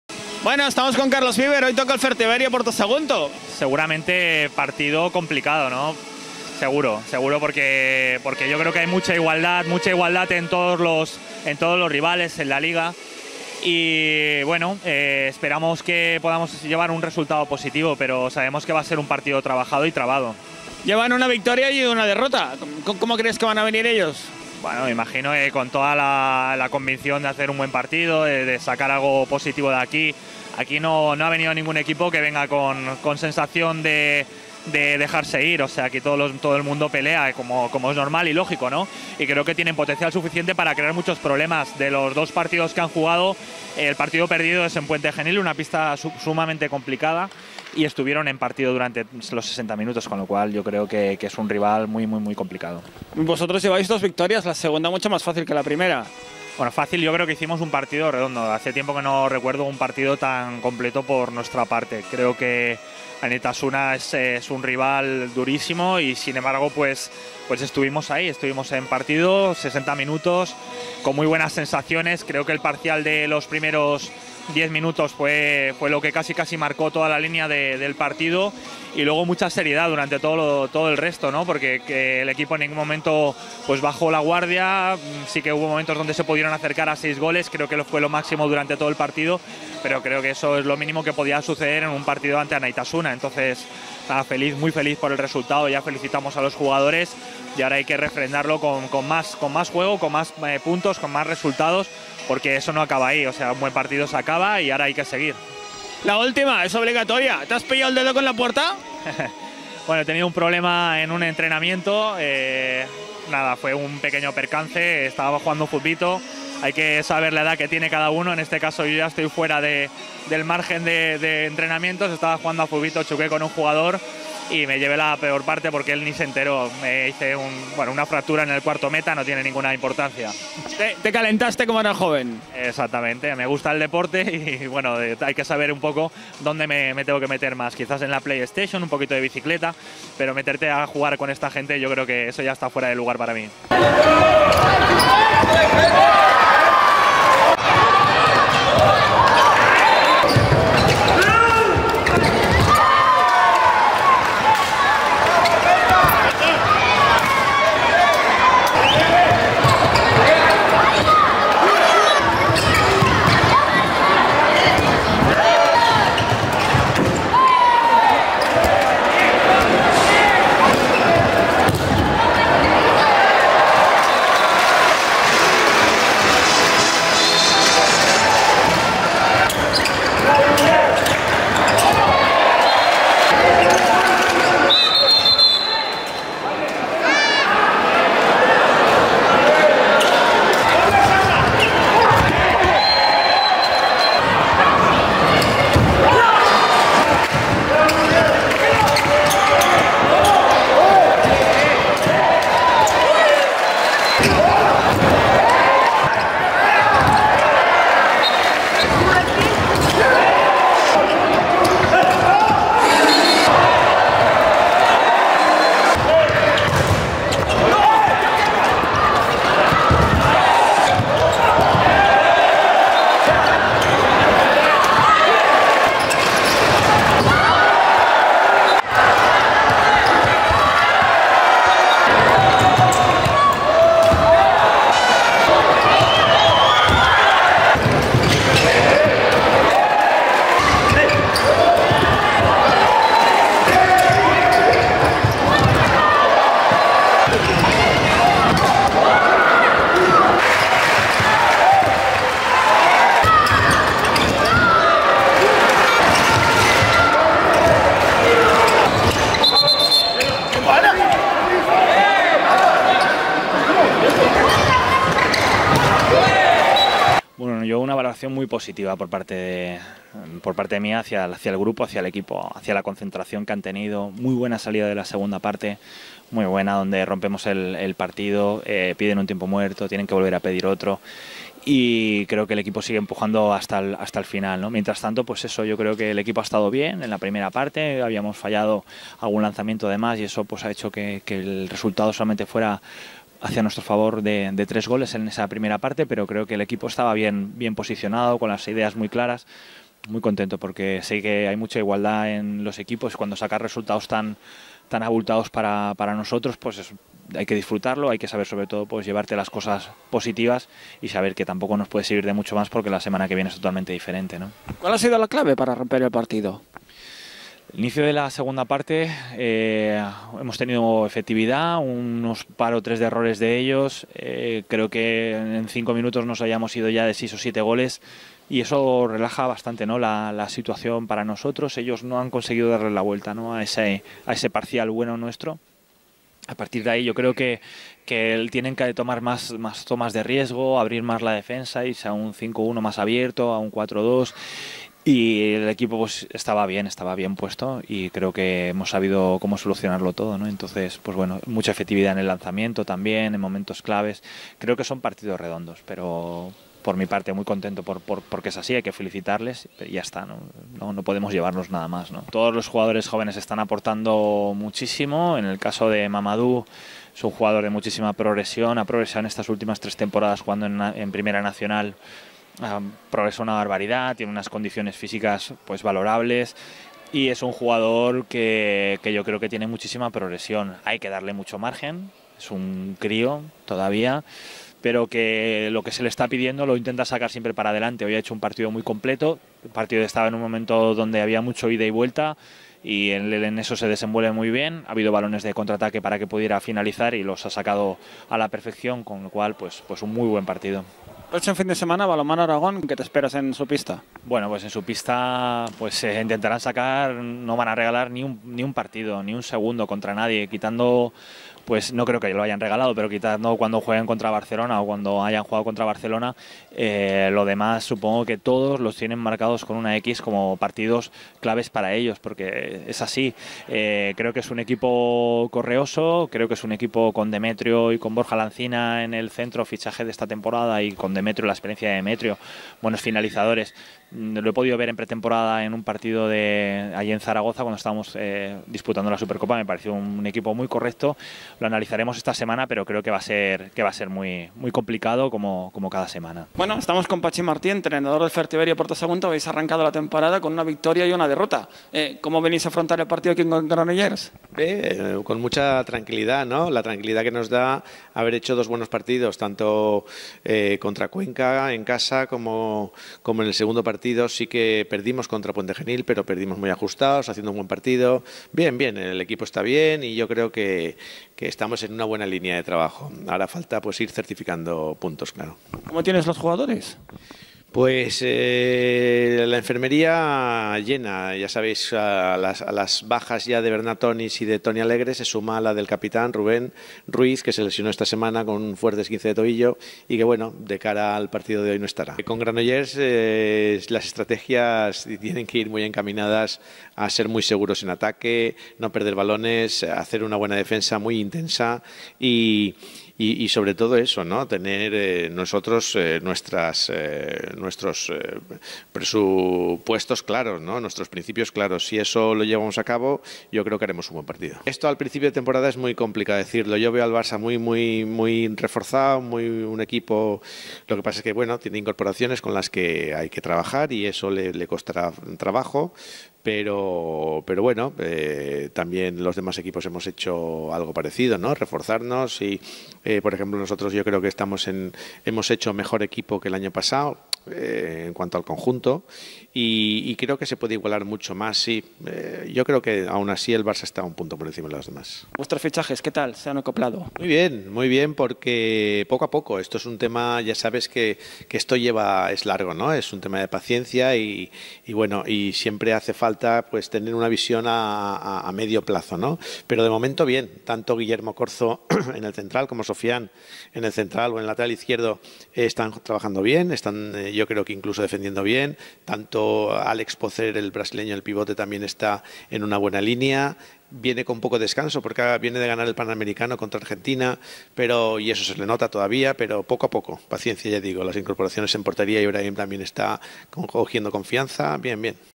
Fraikin BM Granollers - 35 - Fertiberia BM Puerto Sagunto - 20 Liga ASOBAL 2015/16
Este es nuestro reportaje del partido de la liga ASOBAL de balonmano entre el Fraikin BM Granollers y el Fertiberia Puerto Sagunto, con victoria del Granollers por 35 a 20 goles, con las declaraciones de los dos entrenadores a nuestros micrófonos.